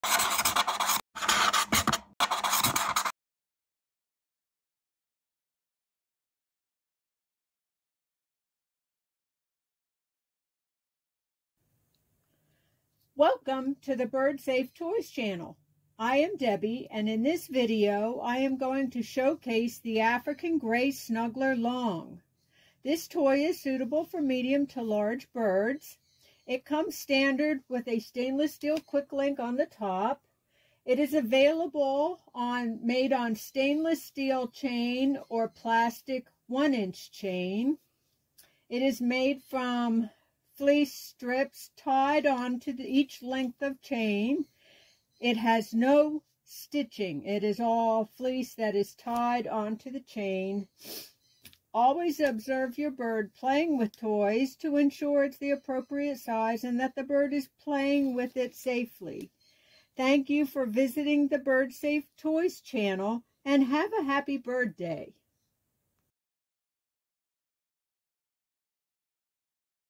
Birdsafe Toys African Grey Snuggler, Long
The Birdsafe Toys African Grey Snuggler, Long is one of our soft, safe fleece birdie-buddies that are the best bird cozies around!  Made to look like an African Grey, this could be your Grey's best friend!  No stitching on Birdsafe Snugglers!